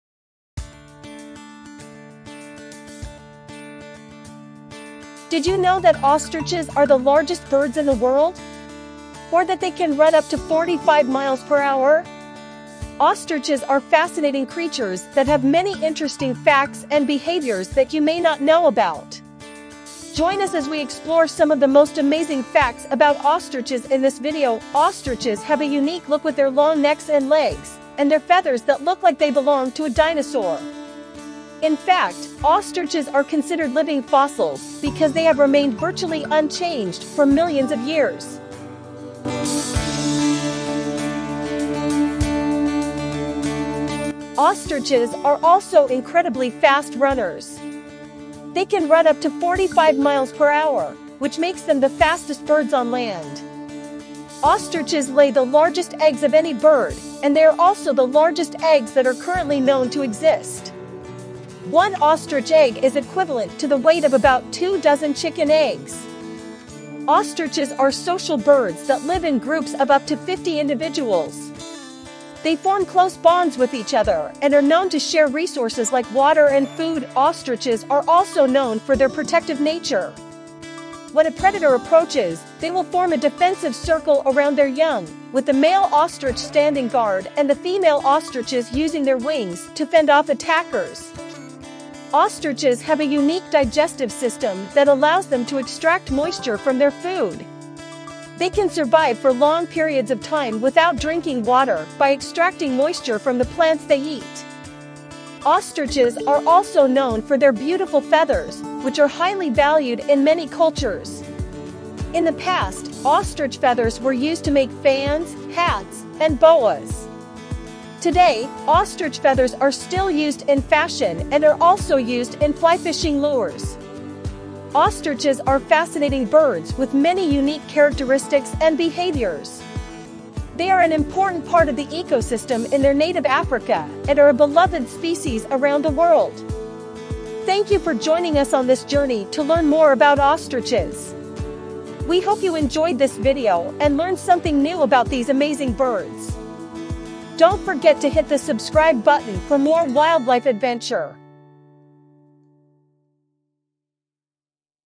All About Ostriches: Interesting Facts and Behaviours - jungle Hug
In this video, we'll explore all about ostriches, the largest bird species on Earth! Did you know that ostriches have three stomachs and can run up to 70 kilometers per hour?

We'll take a closer look at their unique physical characteristics, such as their long necks, powerful legs, and large eyes. You'll also learn about their behaviors, such as how they use their wings for balance and communication, and how they bury their heads in the sand as a defense mechanism.